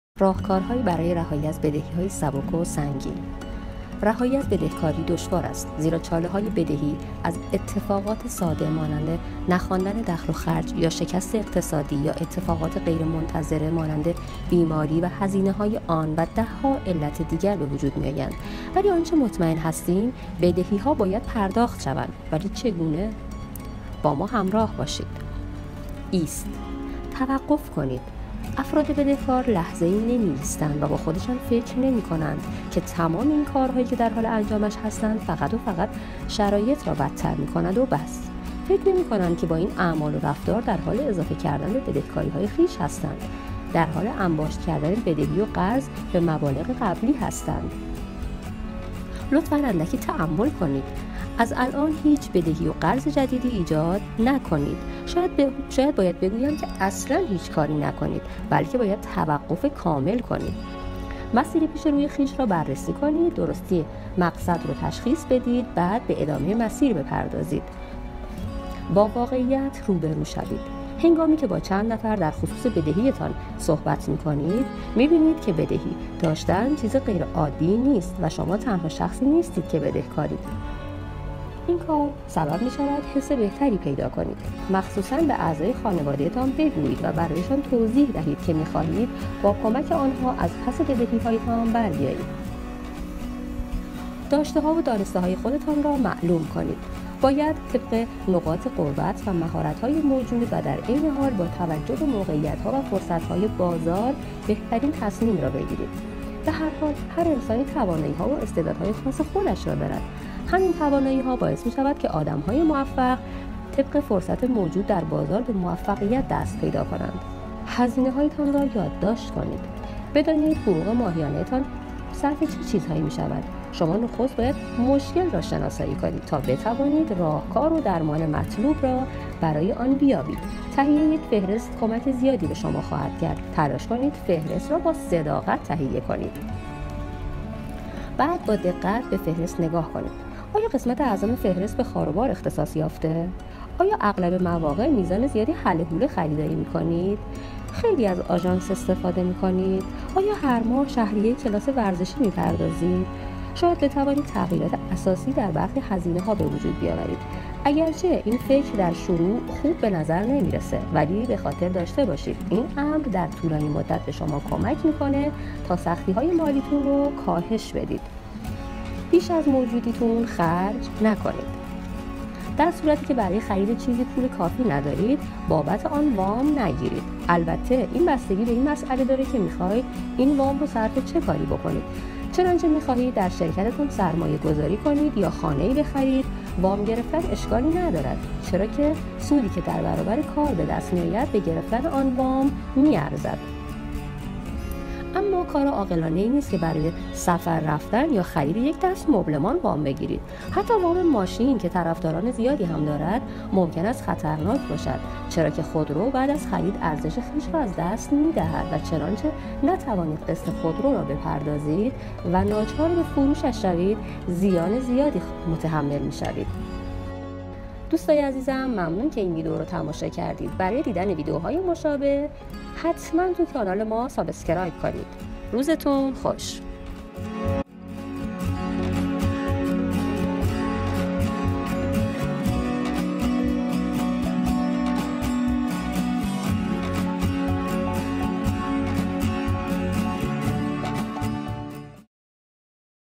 میخواهید از قرضداری و بدهکاری نجات یابید؟
درین ویدیو چندین راهکار برای رهایی از بدهکاری به معرفی گرفته شده است. اگر میخواهید از بدهکاری نجات یابید حتمن این ویدیو را تماشا کنید!